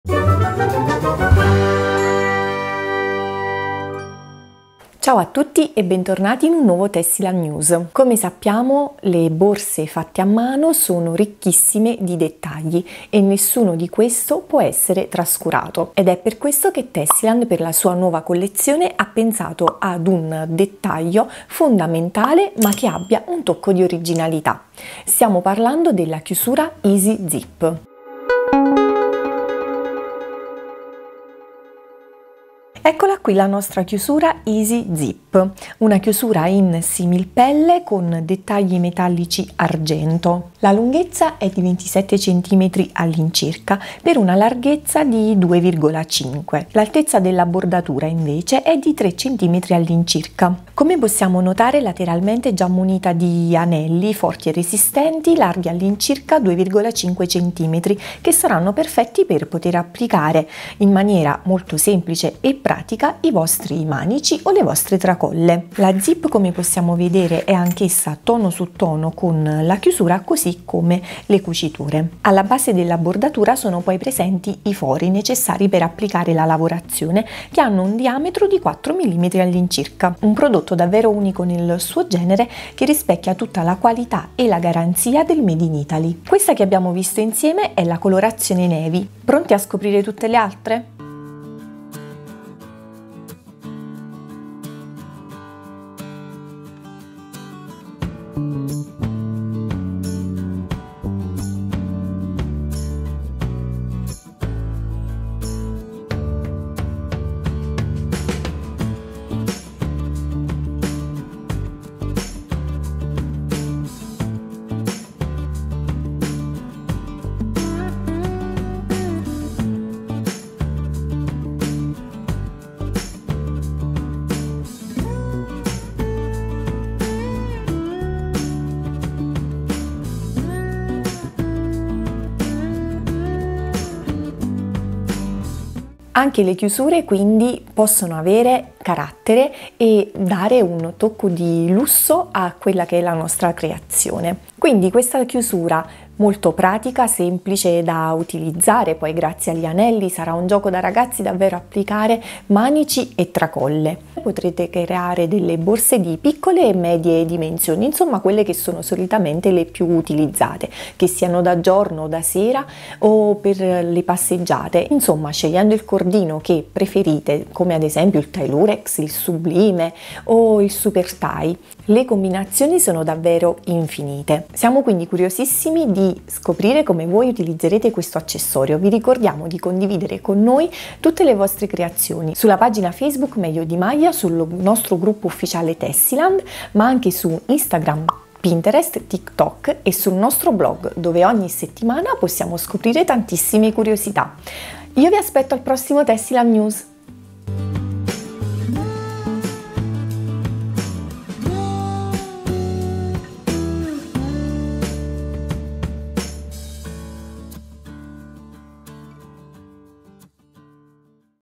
Tessiland News| EASYZIP🌟La chiusura perfetta per le tue pochette a uncinetto 🌟
👜 Design Unico e Raffinato: L'EASYZIP si distingue per la sua raffinatezza. Realizzata in similpelle di alta qualità con dettagli metallici argentati che aggiungono un tocco di eleganza irresistibile. Il suo design sofisticato si completa con una zip interna, che armonizza con il colore esterno della chiusura, dando vita a un connubio estetico affascinante e funzionale.

📏 Dimensioni Ideali: Con una lunghezza di circa 27 cm, una larghezza di 2,5 cm e un’altezza della bordura di circa 3 cm, l’EASYZIP è la chiusura ideale per realizzare pochette di differenti dimensioni e stili. Le sue misure sono studiate per garantire praticità e versatilità in ogni progetto creativo.

💪 Anelli Laterali Resistenti: Gli anelli laterali larghi circa 2,5 cm sono forti e robusti, concepiti per applicare facilmente e in modo sicuro i tuoi manici o tracolle. Un dettaglio che fa la differenza, consentendo di avere una chiusura che non è solo bella da vedere, ma anche comoda da usare e da personalizzare.

🔗 Zip di Qualità: La zip interna a carrarmato con catena da 5mm e un tirante in argento è robusta e scorrevole, garantendo una chiusura sicura e fluida delle tue pochette.

🕳 Fori Perfetti: La bordura è dotata di fori dal diametro di 4mm, posizionati con precisione per facilitare il tuo lavoro a uncinetto, offrendoti la massima libertà creativa.

🇮🇹 Artigianalità Italiana: L’EASYZIP non è un semplice accessorio: è un pezzo unico, frutto dell’artigianalità italiana. Ogni chiusura è realizzata a mano con attenzione e cura per i dettagli, per garantirti un prodotto di qualità superiore, 100% Made in Italy.

💡 Trasforma la Tua Pochette: Con EASYZIP, la tua pochette a uncinetto non sarà più la stessa. Provala ora e lasciati conquistare da un accessorio che combina stile, funzionalità e la qualità inconfondibile del Made in Italy! 🇮🇹🌸